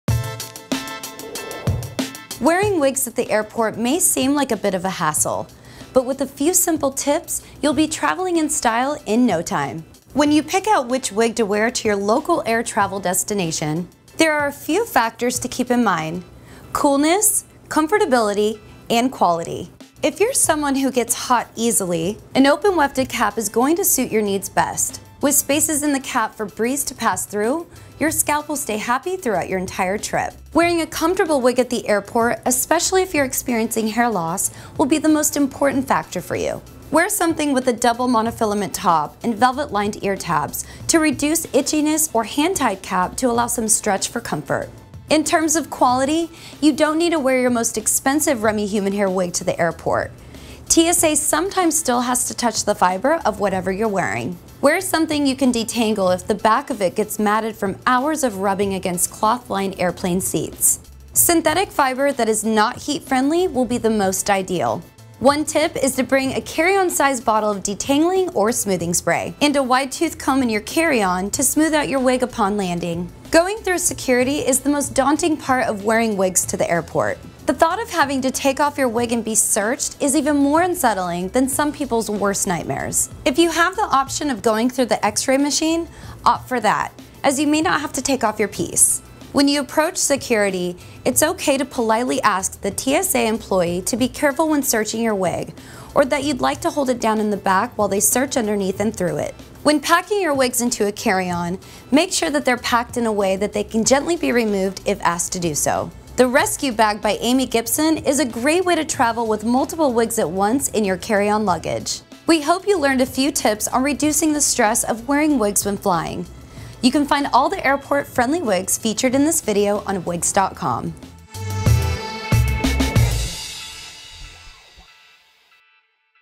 Wearing Wigs at the Airport | Wigs 101
Traveling while wearing wigs can be stressful at first, but make sure you bring a comb and detangler with you in your carry on for on-the-go style touchups. See the styles and products featured below!

STYLES & PRODUCTS FEATURED:
0:03 - Miranda by Jon Renau in color 12FS8
0:23 - Miranda by Jon Renau in color 12FS8
0:37 - Camilla by Jon Renau in color 12FS8
1:09 - HD Smooth Detangler by Jon Renau
1:10 - Wide Tooth Comb by BeautiMark
1:46 - ResQ Bag by Amy Gibson

FORUM:
Join our wig community to share your stories & experiences, help others, and be a part of the family!